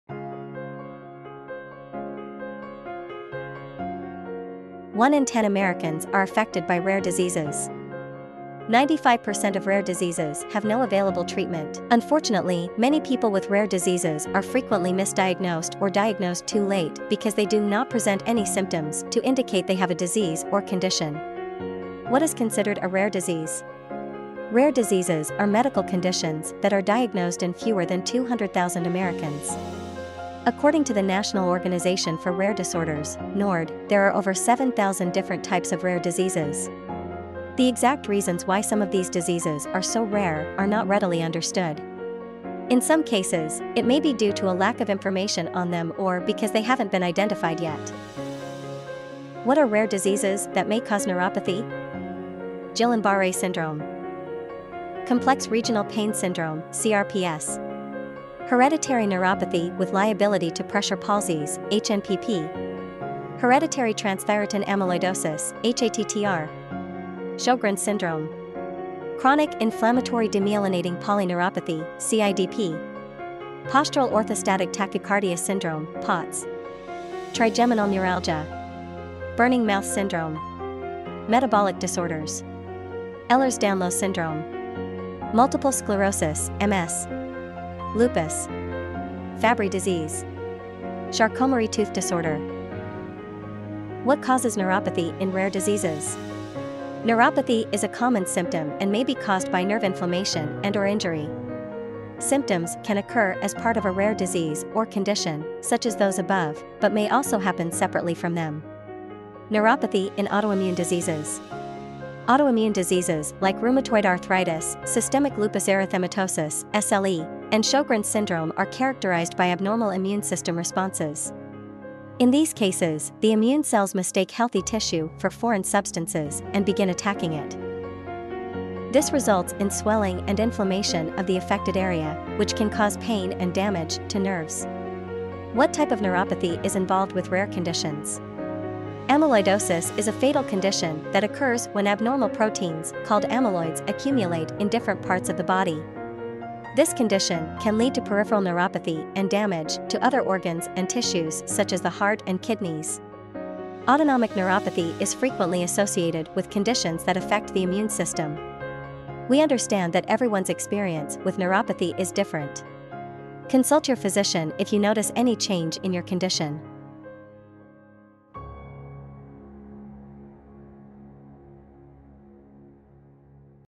Rare Diseases that cause Peripheral Neuropathy
Rare diseases affect 1 in 10 Americans on average and 95% of them have no available treatment. Unfortunately, many people with rare diseases are frequently misdiagnosed or diagnosed too late because they do not present any symptoms to indicate they have a disease or condition.

Let's discuss what makes a rare condition, what causes neuropathy to be a symptom in these cases, and the type of neuropathy often associated with each of these rare conditions